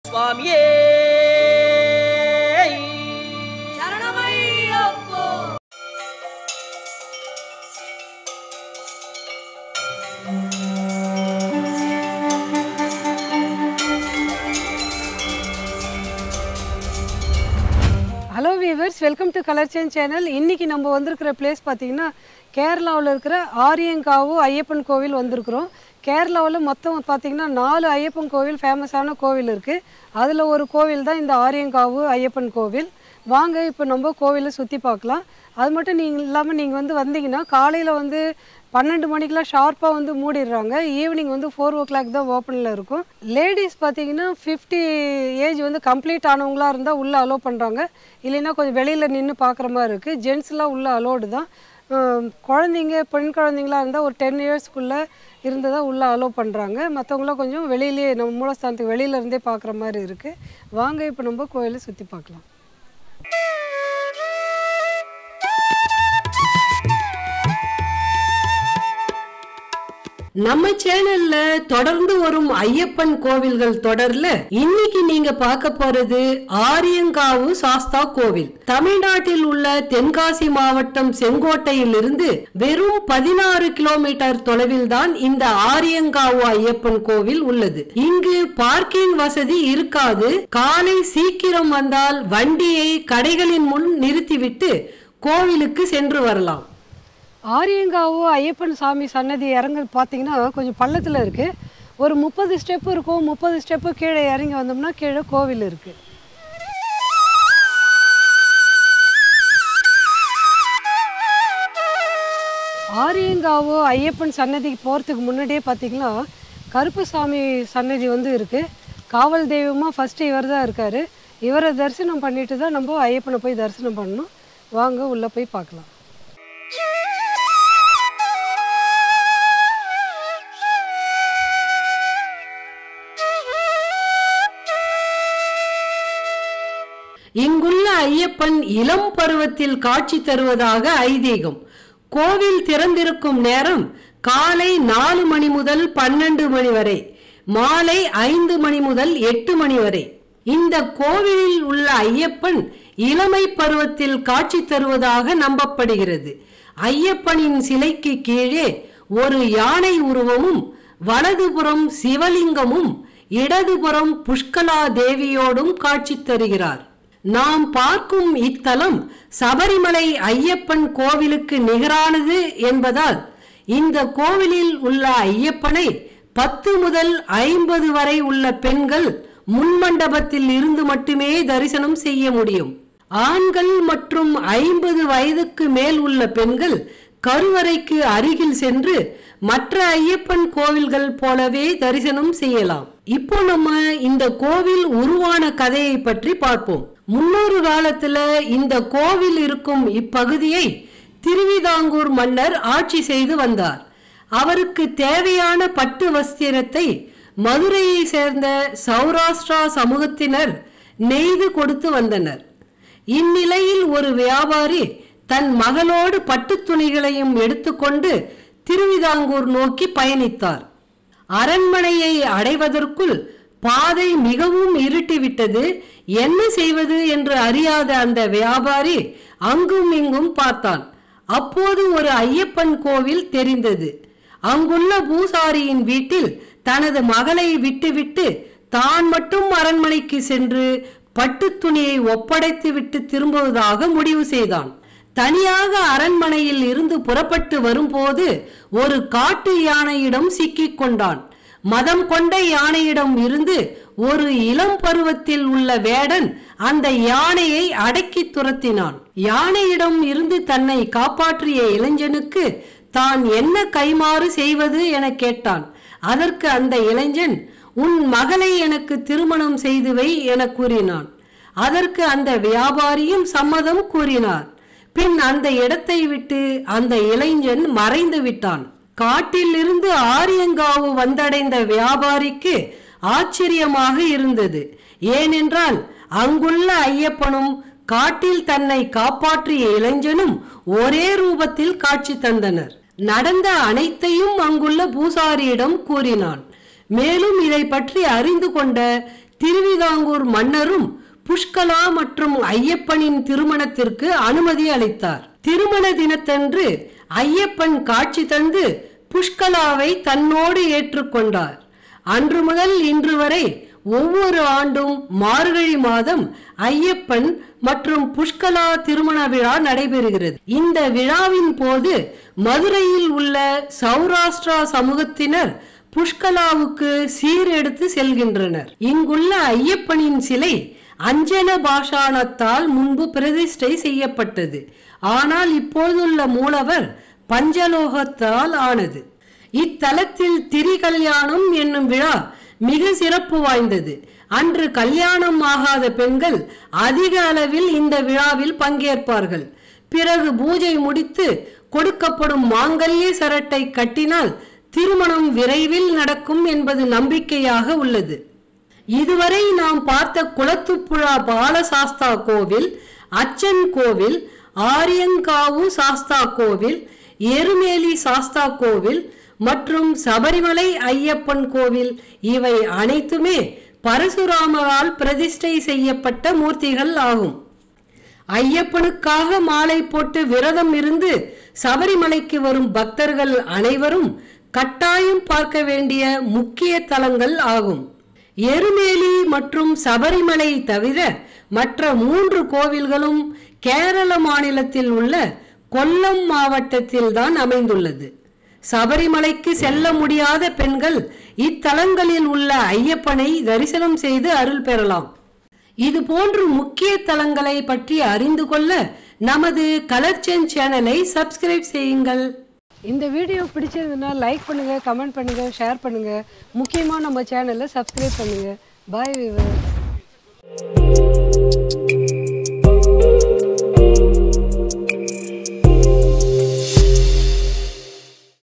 Aryankavu Ayyappan Temple | Kollam-Kerala | Dharma Sastha Temple | Sabarimala Travel
The Aryankavu Sastha Temple is a Hindu temple located in Aryankavu in the Kollam district of the Indian state of Kerala. It is one of the five major temples dedicated to Shasta in Kerala, the others being Kulathupuzha, Achankovil, Sabarimala and Kanthamala. The temple's idol is believed to have been consecrated by Parashurama. It is located in Aryankavu in the tehsil of Punalur in Kollam district. The temple lies near the border of Tamil Nadu on National Highway 744, about 73 km (45 mi) from Kollam, 33 km (21 mi) from Punalur and 21 km (13 mi) from Tenkasi.